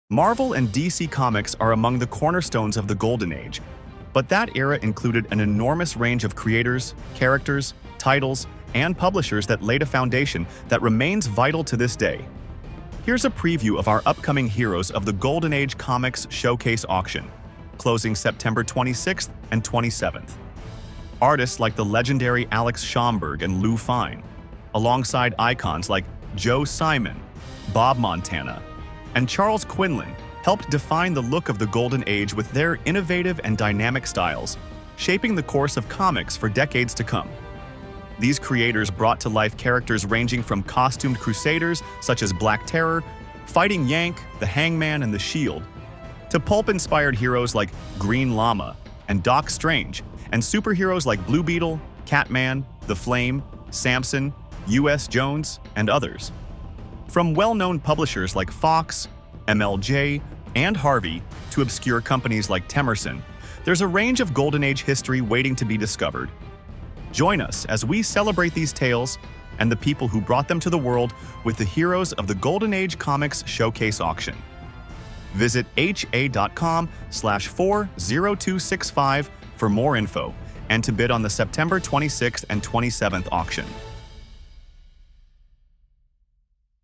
Heroes from the Golden Age of Comics Featured at Heritage in September
Marvel and DC Comics are among the cornerstones of the Golden Age, but that era included an enormous range of creators, characters, titles, and publishers that laid a foundation that remains vital to this day.

Artists like the legendary Alex Schomburg and Lou Fine; alongside icons like Joe Simon, Bob Montana, Irv Novick, and Charles Quinlan helped define the look of the Golden Age with their innovative and dynamic styles, shaping the course of comics for decades to come.

Join us as we celebrate these tales and the people who brought them to the world with the Heroes of the Golden Age Comics Showcase Auction.